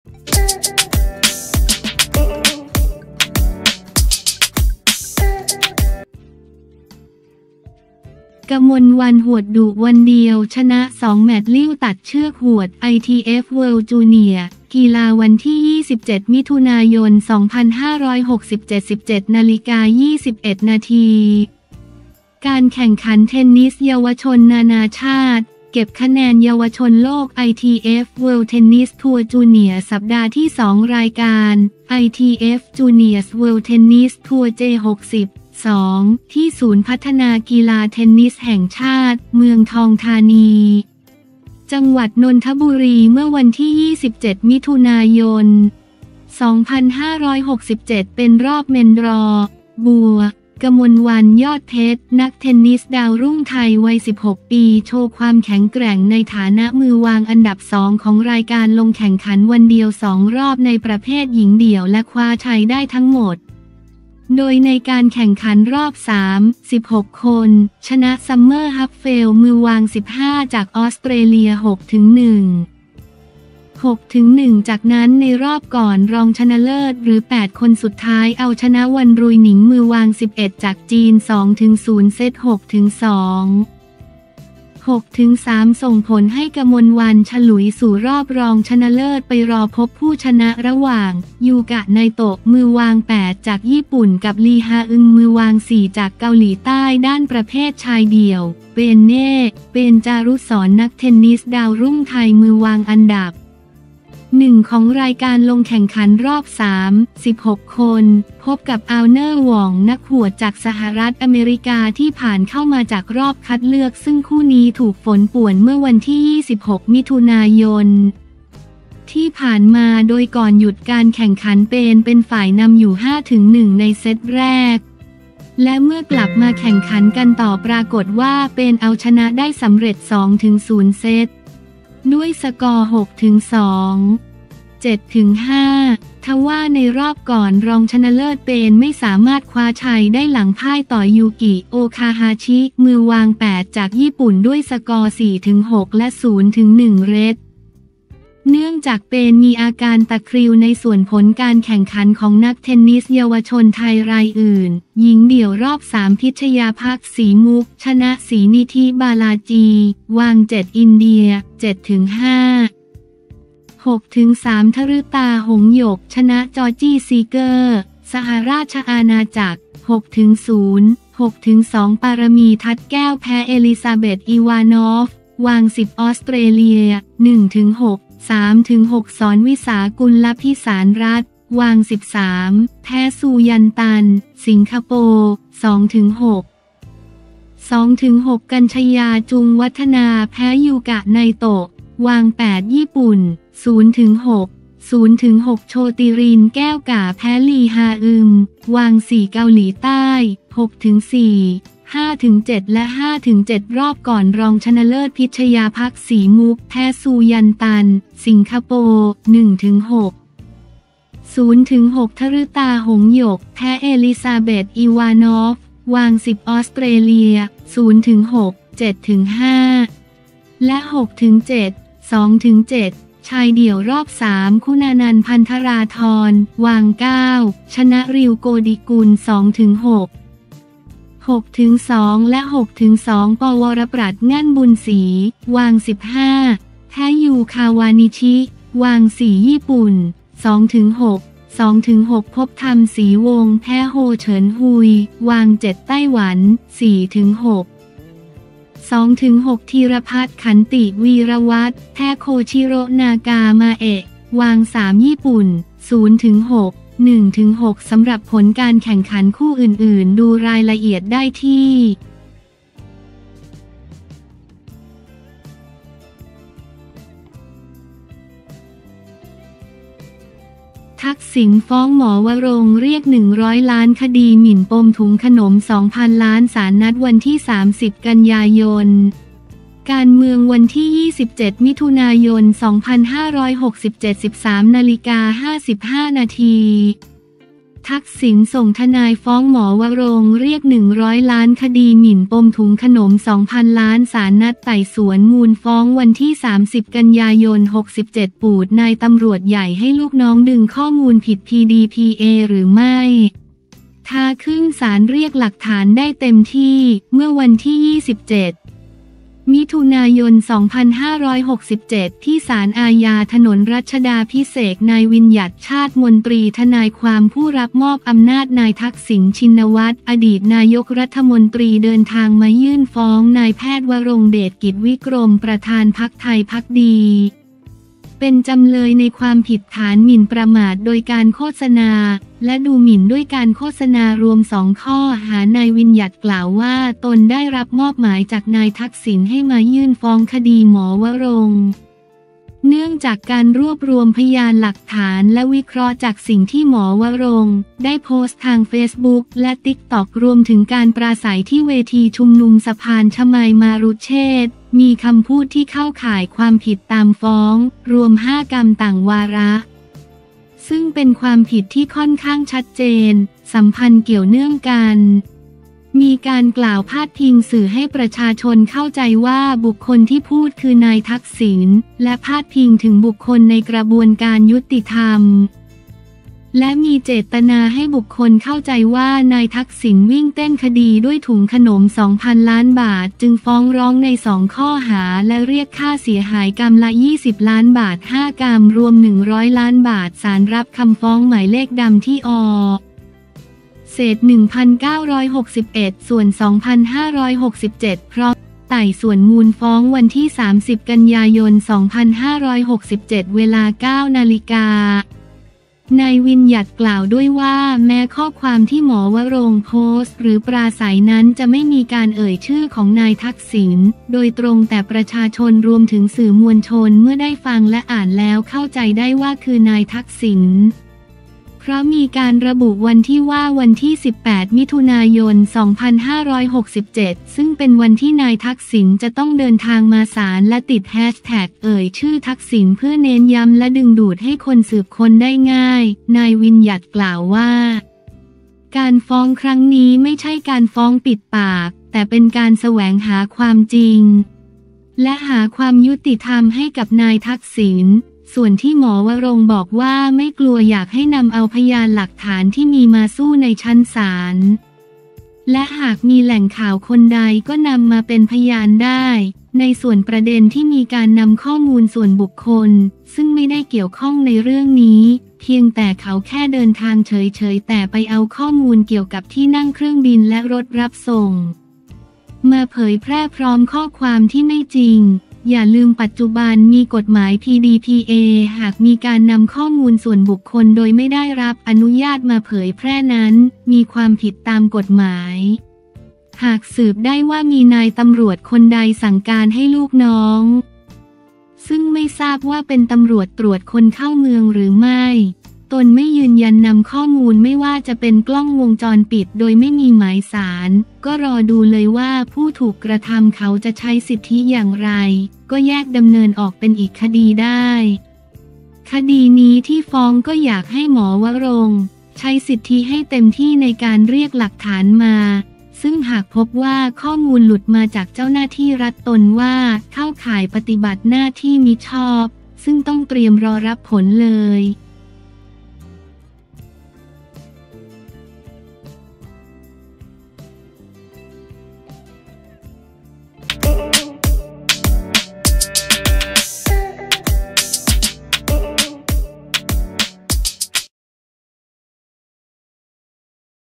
กมลวรรณ หวดดุ วันเดียวชนะ 2 แมทช์ ลิ่วตัดเชือกหวดไอทีเอฟเวิลด์จูเนียร์
กีฬา
27 มิ.ย. 2567
17:21 น.
การแข่งขันเทนนิสเยาวชนนานาชาติ เก็บคะแนนเยาวชนโลก ไอทีเอฟ เวิลด์ เทนนิส ทัวร์ จูเนียร์ สัปดาห์ที่ 2 รายการ “ไอทีเอฟ จูเนียร์ส เวิลด์ เทนนิส ทัวร์ เจ60 (2)” ที่ศูนย์พัฒนากีฬาเทนนิสแห่งชาติ เมืองทองธานี จ.นนทบุรี เมื่อวันที่ 27 มิ.ย. 2567 เป็นรอบเมนดรอว์

“บัว” กมลวรรณ ยอดเพ็ชร นักเทนนิสดาวรุ่งไทยวัย 16 ปี โชว์ความแข็งแกร่งในฐานะมือวางอันดับ 2 ของรายการ ลงแข่งขันวันเดียว 2 รอบในประเภทหญิงเดี่ยวและคว้าชัยได้ทั้งหมด โดยในการแข่งขันรอบสาม (16 คน) ชนะ ซัมเมอร์ ฮับเฟลด์ มือวาง 15 จากออสเตรเลีย 6-1, 6-1 จากนั้นในรอบก่อนรองชนะเลิศ หรือ 8 คนสุดท้าย เอาชนะ วัน รุยหนิง มือวาง 11 จากจีน 2-0 เซต 6-2, 6-3 ส่งผลให้ กมลวรรณ ฉลุยสู่รอบรองชนะเลิศไปรอพบผู้ชนะระหว่าง ยูกะ ไนโตะ มือวาง 8 จากญี่ปุ่น กับ ลี ฮา อึม มือวาง 4 จากเกาหลีใต้

ด้านประเภทชายเดี่ยว “เปนเน่” เปน จารุศร นักเทนนิสดาวรุ่งไทย มือวางอันดับ 1 ของรายการ ลงแข่งขันรอบสาม (16 คน) พบกับ อาวเนอร์ หว่อง นักหวดจากสหรัฐฯ ที่ผ่านเข้ามาจากรอบคัดเลือก ซึ่งคู่นี้ถูกฝนป่วนเมื่อวันที่ 26 มิ.ย. ที่ผ่านมา โดยก่อนหยุดการแข่งขัน เปน เป็นฝ่ายนำอยู่ 5-1 ในเซตแรก และเมื่อกลับมาแข่งขันกันต่อ ปรากฏว่า เปน เอาชนะได้สำเร็จ 2-0 เซต ด้วยสกอร์ 6-2, 7-5 ทว่า ในรอบก่อนรองชนะเลิศ เปน ไม่สามารถคว้าชัยได้ หลังพ่ายต่อ ยูกิ โอคาฮาชิ มือวาง 8 จากญี่ปุ่น ด้วยสกอร์ 4-6 และ 0-1Ret. เนื่องจาก เปน มีอาการตะคริว

ในส่วนผลการแข่งขันของนักเทนนิสเยาวชนไทยรายอื่น หญิงเดี่ยว รอบสาม พิชญาภัค ศรีมุกข์ ชนะ ศรีนิธิ บาลาจี (วาง 7-อินเดีย) 7-5, 6-3 ธฤตา หงษ์หยก ชนะ จอร์จี้ ซีเกอร์ (สหราชอาณาจักร) 6-0, 6-2 ปารมี ทัดแก้ว แพ้ เอลิซาเบธ อิวานอฟ (วาง 10-ออสเตรเลีย) 1-6, 3-6 ศรร์วิศา กุลพิศาลรัศม์ (วาง 13) แพ้ ซู ยัน ตัน (สิงคโปร์) 2-6, 2-6 กัญจน์ชญา จูงวัฒนา แพ้ ยูกะ ไนโตะ (วาง 8-ญี่ปุ่น) 0-6, 0-6 โชติรินทร์ แก้วก่า แพ้ ลี ฮา อึม (วาง 4-เกาหลีใต้) 6-4, 5-7 และ 5-7

รอบก่อนรองชนะเลิศ พิชญาภัค ศรีมุกข์ แพ้ ซู ยัน ตัน (สิงคโปร์) 1-6, 0-6 ธฤตา หงษ์หยก แพ้ เอลิซาเบธ อิวานอฟ (วาง 10-ออสเตรเลีย) 0-6, 7-5 และ 6-7(2-7)
ชายเดี่ยว รอบสาม คุณานันท์ พันธราธร (วาง 9) ชนะ ริว โกฎิกุล 2-6, 6-2 และ 6-2 ปวรปรัชญ์ งั่นบุญศรี (วาง 15) แพ้ ฮยู คาวานิชิ (วาง 4-ญี่ปุ่น) 2-6, 2-6 ภพธรรม ศรีวงษ์ แพ้ โฮ เฉิน ฮุย (วาง 7-ไต้หวัน) 4-6, 2-6 ธีรภัทร ขันติวีรวัฒน์ แพ้ โคชิโระ นากามาเอะ (วาง 3-ญี่ปุ่น) 0-6, 1-6
สำหรับผลการแข่งขันคู่อื่น ๆ ดูรายละเอียดได้ที่